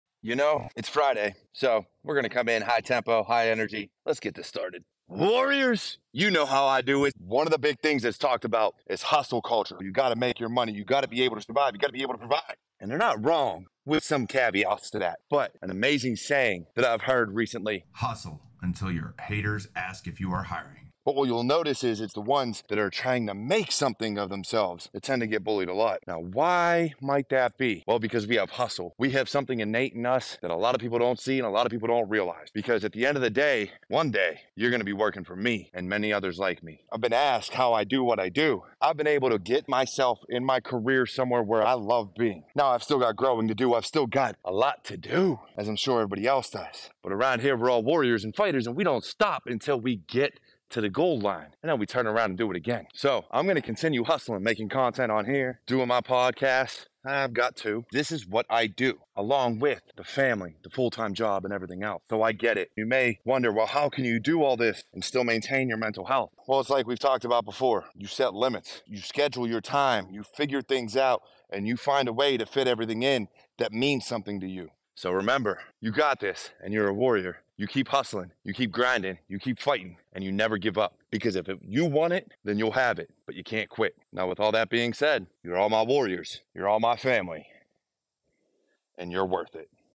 Hustle Beyond Haters: When They Ask if You're Hiring
Welcome to our latest video where we dive into the world of relentless hustle and undeniable success! In this short but impactful clip, we explore the empowering concept of putting in the hard work until your haters are left in awe, asking if you're hiring for your journey to the top.

Life is full of challenges, and there will always be skeptics and naysayers. But rather than letting their doubts hold us back, we can channel our energy into hustling, improving, and achieving our goals. Join us as we break down the idea that your determination and dedication can become so evident that even your critics start to take notice.

Through real-life examples and motivational insights, we'll inspire you to rise above negativity, embrace the grind, and turn your aspirations into reality. If you're ready to let your success do the talking and make your journey an undeniable testament to your hard work, don't miss this video.

It's time to hustle until your haters ask if you're hiring – let's make success the best response to doubt!